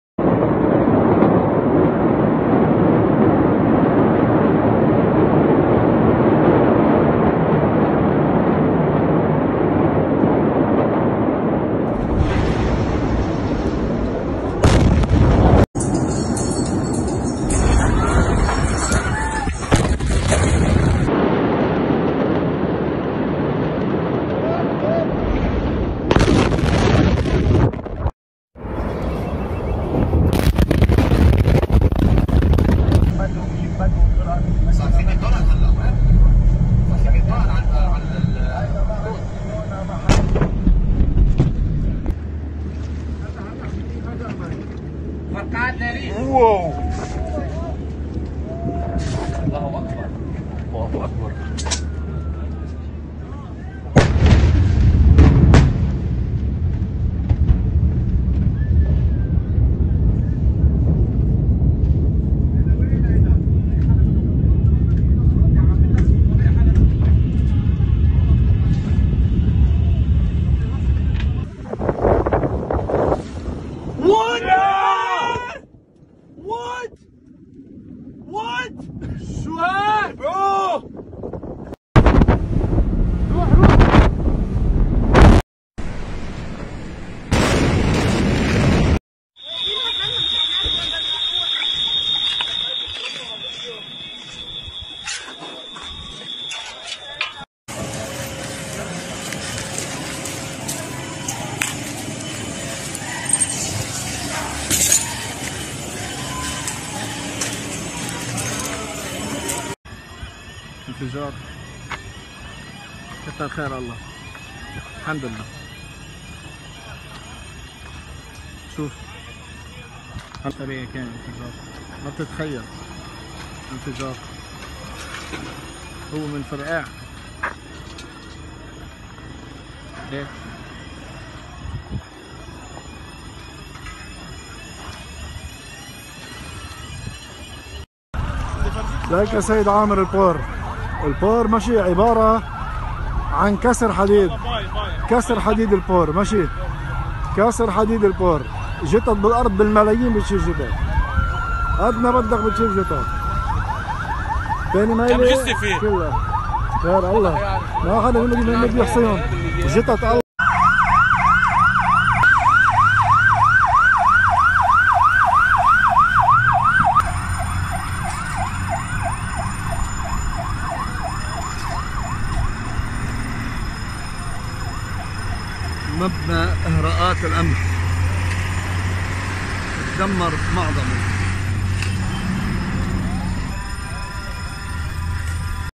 20200804 - 黎巴嫩貝魯特大爆炸最新影片(合輯)
貝魯特港區一處存放2750噸硝酸銨的倉庫昨天發生大爆炸，死傷慘重，已知造成逾4000人受傷、逾百人喪生。
爆炸產生的超強震波波及半個貝魯特市，無數建築物慘遭摧毀，城市地面也佈滿了震碎的玻璃碎片與坍塌的建築殘骸。